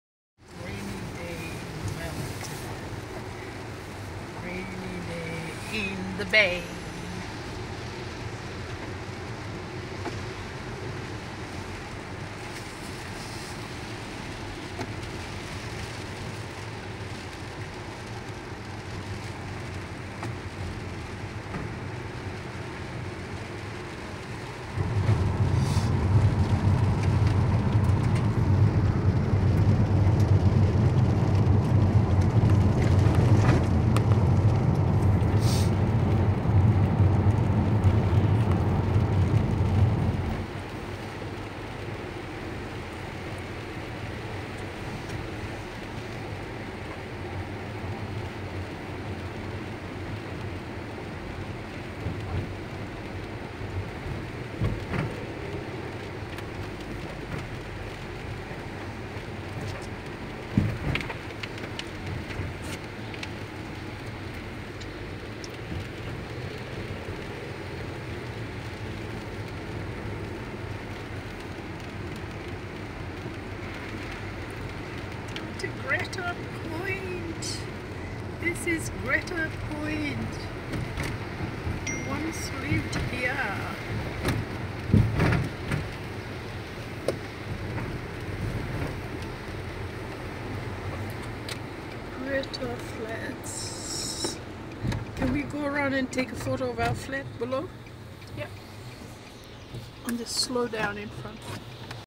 Driving In the Rain to Greta Point Wellington NZ.
Rainy afternoon today, Sunday 13 August, 2017.